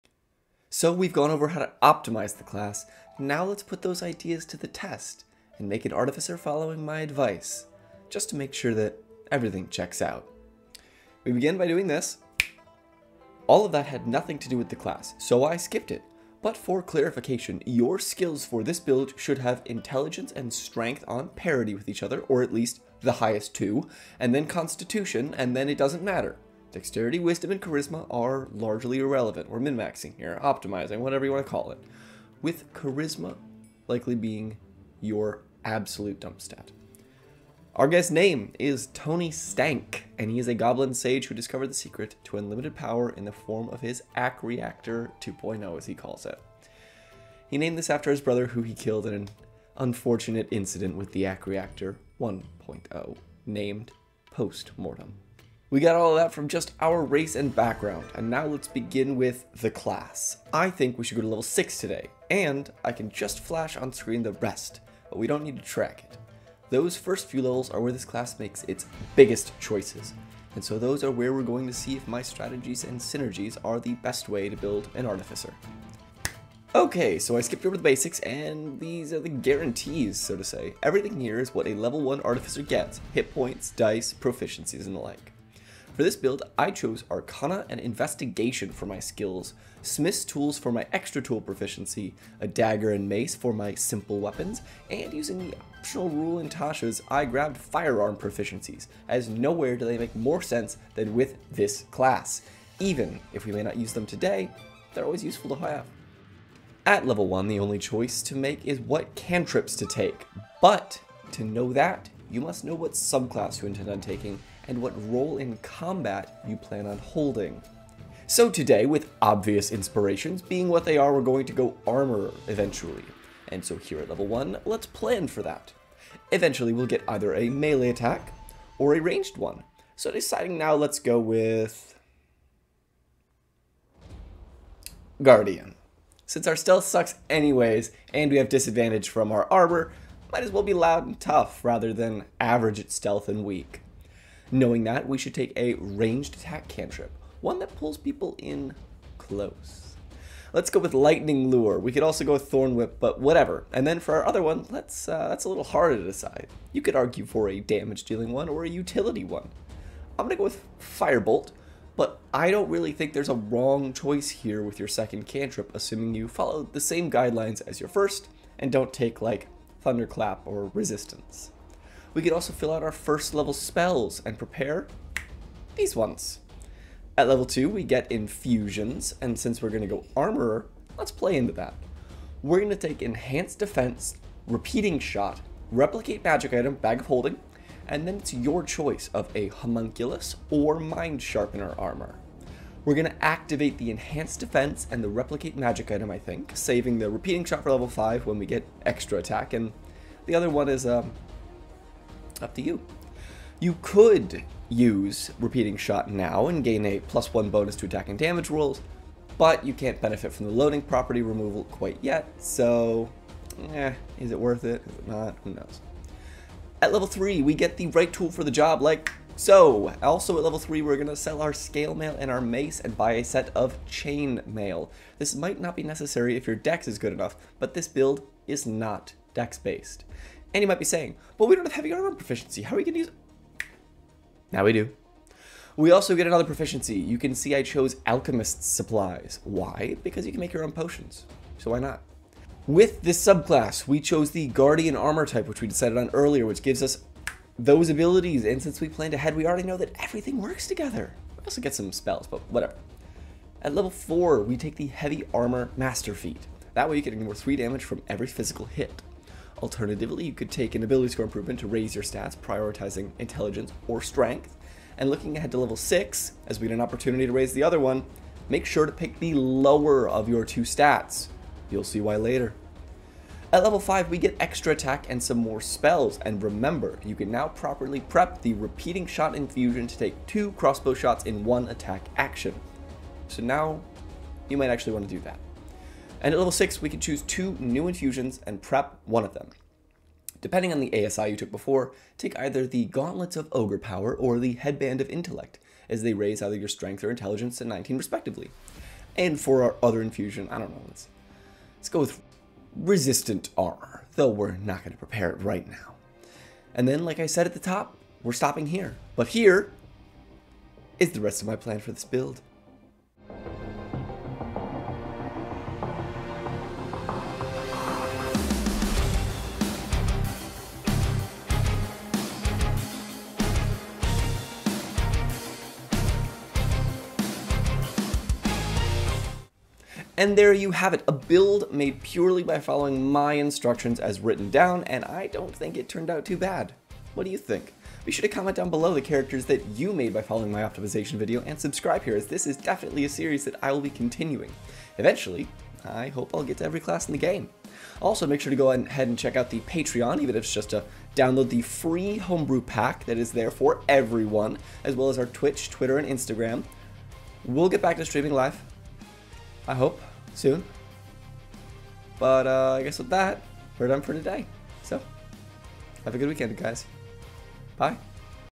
Bite-Sized Builds - The Invincible Artificer (D&D 5e)
Last time we talked about how to optimize artificers, now let's use those rules and make one of our own.

Be sure to head over to our twitch page to catch our weekly games

Timestamps
0:00 - Intro
0:11 - Pre-Class Formalities
1:15 - Level 1: The Basics and Planning Ahead
2:55 - Level 2: Infusions
3:33 - Level 3: Subclass
4:13 - Level 4: ASI or Feat?
4:33 - Level 5: Boring Auto-Grab Things
4:46 - Level 6: More Infusions!
5:11 - Leveling Up Further
5:30 - Outro